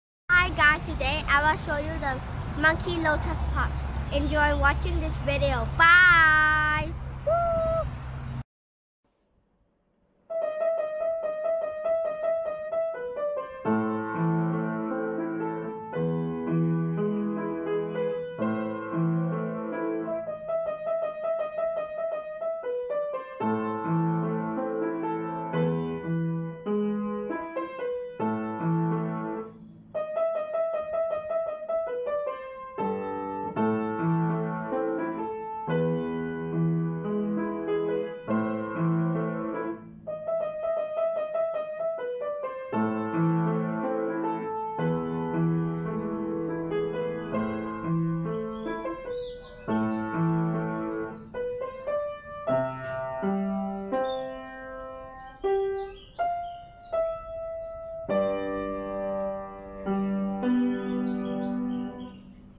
Lotus, lotus pods, monkey lotus pods, monkey ดอกบัว ฝักบัว ลิง ฝักบัวลิง | Taytan Kant
Lotus, lotus pods, monkey, monkey lotus pods, beautiful lotus pods.  Taytan take to see a lotus or lotus pods, that nature creates like a monkey face is monkey lotus pods, It's a beautiful lotus pods or beautiful monkey lotus pods, miracle of nature. (Piano by Taytan)
ดอกบัว ฝักบัว ลิง ฝักบัวลิง เตตั้นพาดู บัว หรือ ฝักบัว ซึ่งธรรมชาติสร้างสรรค์ คล้ายหน้าลิง เป็น ฝักบัวลิง เป็นฝักบัวที่สวยงาม หรือ ฝักบัวลิงที่สวยงาม มหัศจรรย์ธรรมชาติจริง ๆ